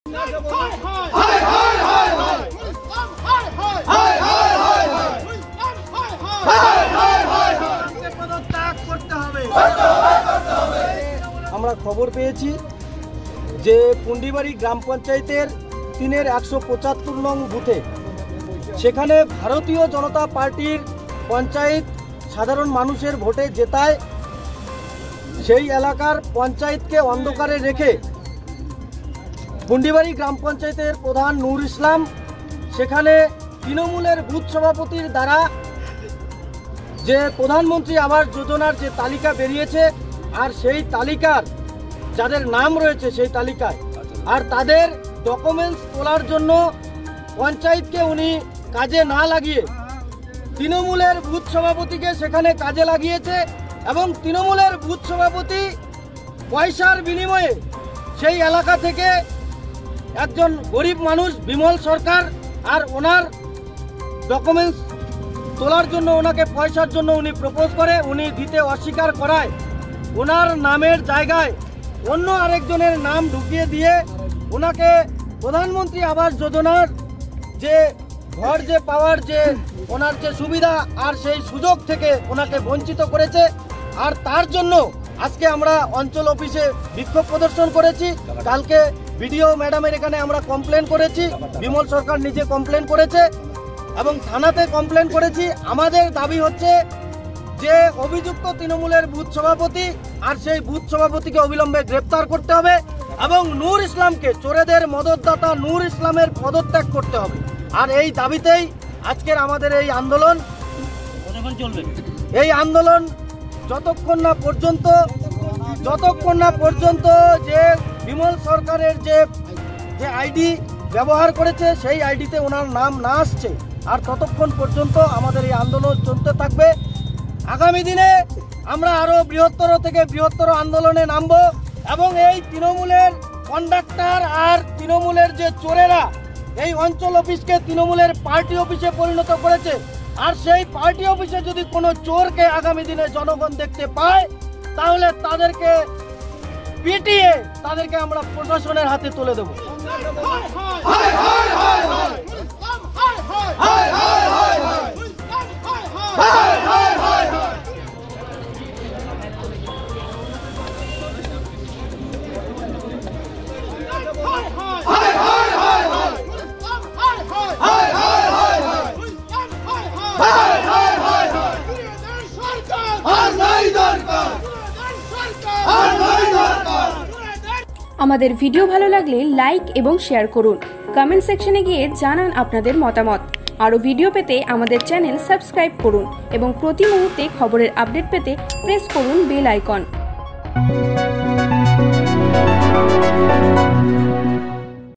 আবাস যোজনায় দুর্নীতির অভিযোগে পুন্ডিবাড়িতে বিজেপির তুমুল বিক্ষোভ
আবাস যোজনায় দুর্নীতির অভিযোগ বিজেপির বিক্ষোভ। তৃণমূলের প্রধান ও বুথ সভাপতি বিরুদ্ধে দুর্নীতির অভিযোগ। তৃণমূলের বুথ সভাপতি প্রাপকদের বঞ্চিত করে টাকার বিনিময়ে ঘর দিচ্ছেন। এই অভিযোগে, পুন্ডিবাড়ি সোনারপুর রোড পথ অবরোধ করে বিজেপি। অবিলম্বে প্রধানের পদত্যাগের দাবি করেছে বিজেপি কর্মীরা। সমস্ত অভিযোগ অস্বীকার করেছেন প্রধান নুর ইসলাম।